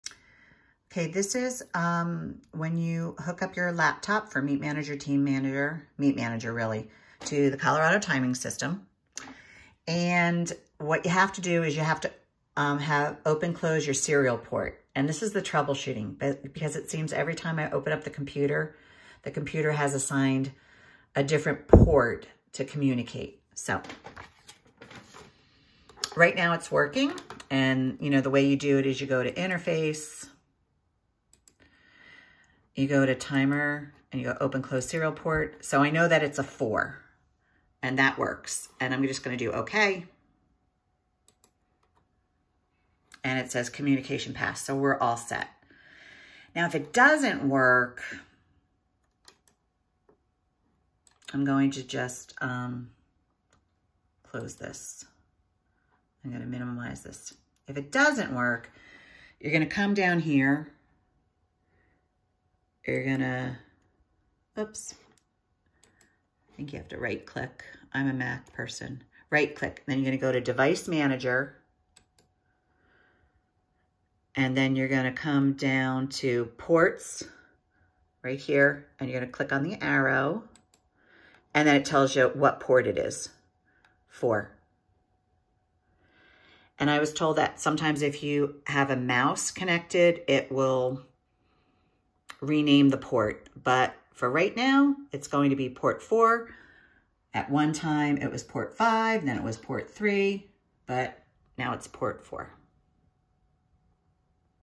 4.Hy-tek Swimming MEET Manager COMMUNICATING with the Colorado Timing System .
Hy-tek Swimming MEET Manager needs to COMMUNICATE with the CTS in order to pull the times in. If communication fails it is usually because the wrong port is open. The serial/port is any number from 0-10. Mine has reset itself so many times. If a mouse is attached to the laptop that can throw it off as well. This troubleshoots if you can’t communicate with the CTS